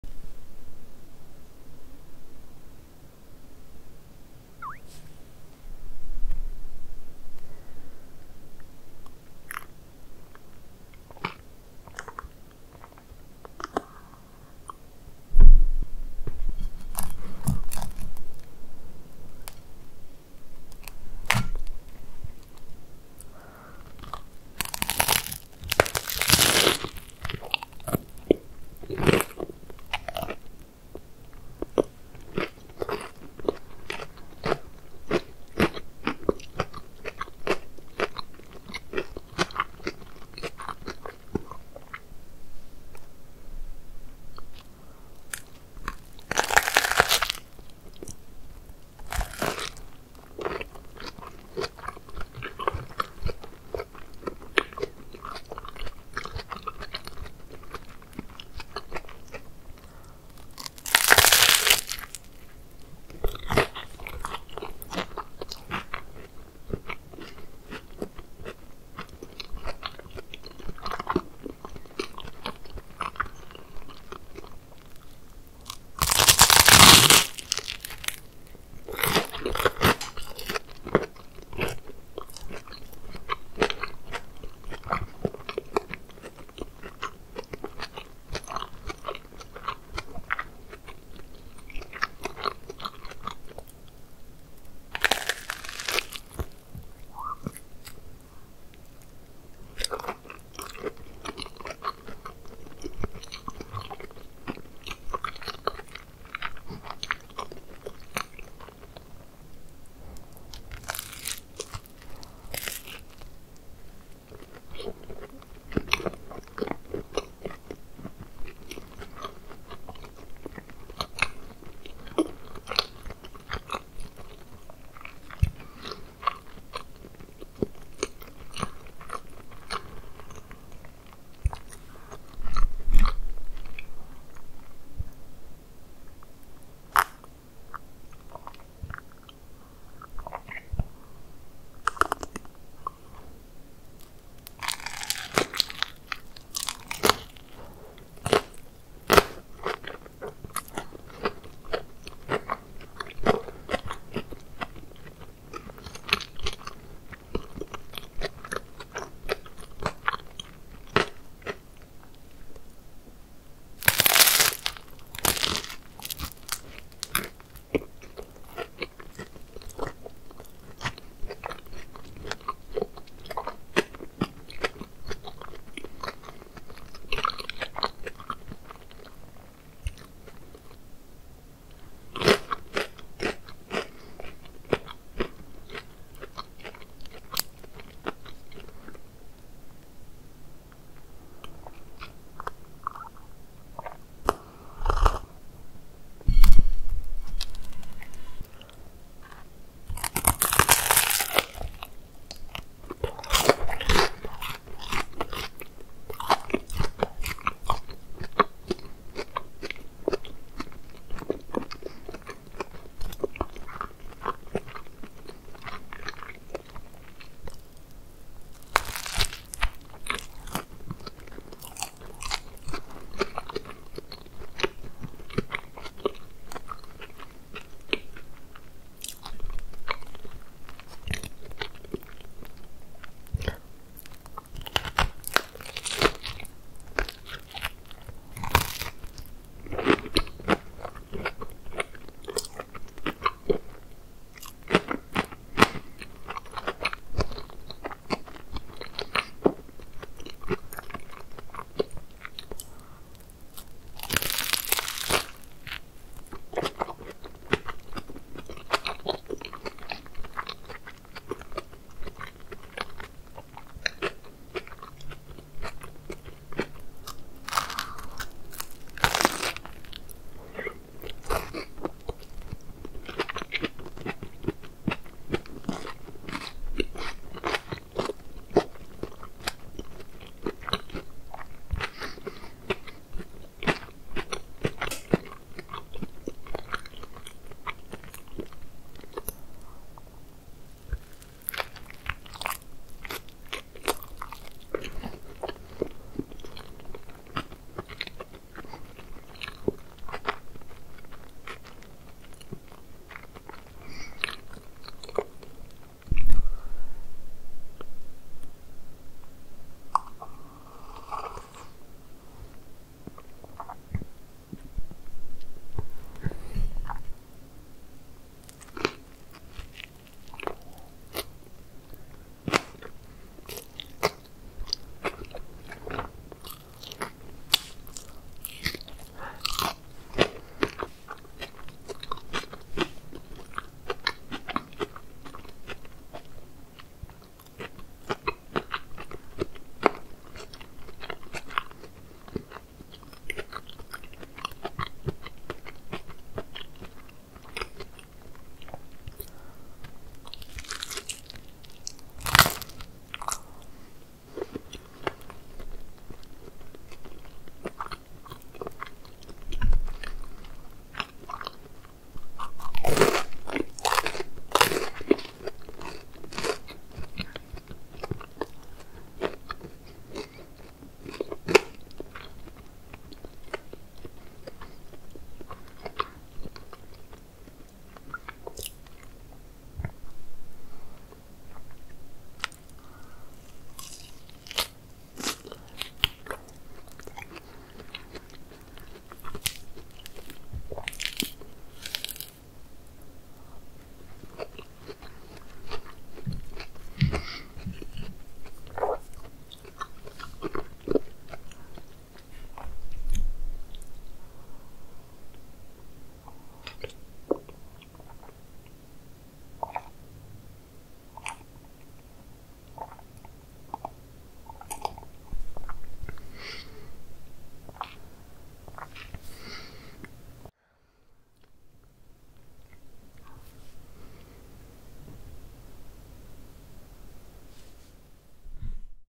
たっぷり卵のふわカリピザトーストつくって食べた【咀嚼音・ASMR】【字幕】
※咀嚼音、食べている口元が苦手な方はご注意ください。

喫茶店「コメダ珈琲」の大好きなメニューを
参考に、おうちで作ってみちゃいました。
サクサクのフワフワでとっても美味しかった😋